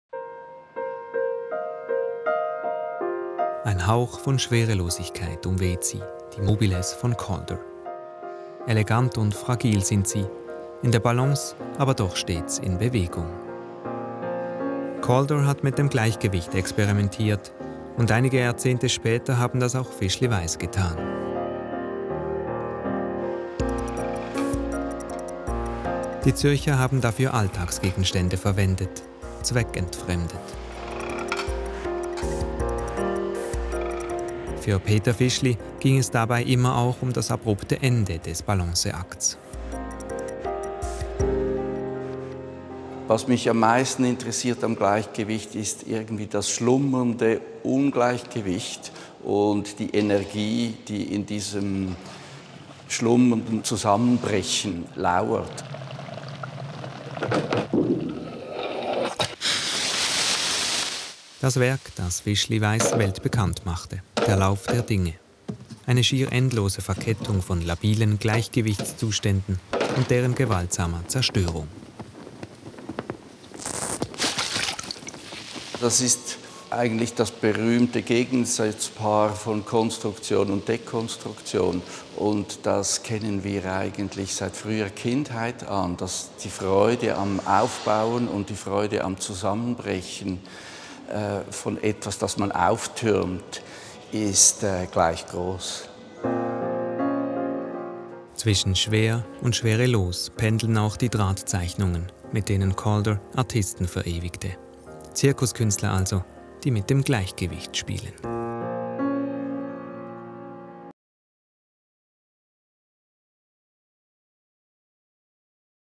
Fischli-Weiss and Calder works on show
Delicate artworks by the Swiss artist duo Fischli/Weiss and the American Alexander Calder are on show at the Fondation Beyeler. They play with balance. (SRF/swissinfo.ch)